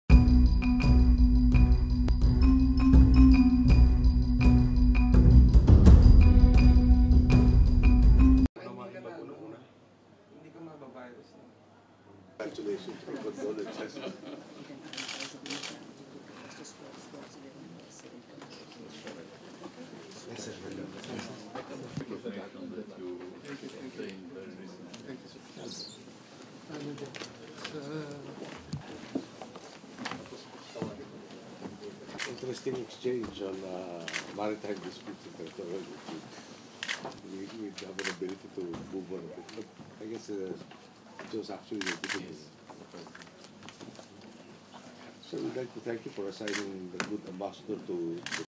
Bilateral Meeting with Prime Minister Mario Monti of Italy
National Convention Centre, Vientiane, Lao PDR
6 November 2012

President Benigno S. Aquino III held his final bilateral meeting with Italian Prime Minister Mario Monti on the last day of the 9th Asia-Europe Meeting (ASEM 9) in Vientiane, Lao PDR.

In their scheduled meeting right after Plenary Session V at the National Convention Centre, the two leaders focused on enhancing bilateral relations, particularly in the area of defense and finance.

Europe is host to 746,701 Filipinos whose remittances amounted to US$2.1 billion in 2011. In Italy alone, there are an estimated 134,154 Filipinos as of October 2011. They constitute the sixth largest migrant community in the country.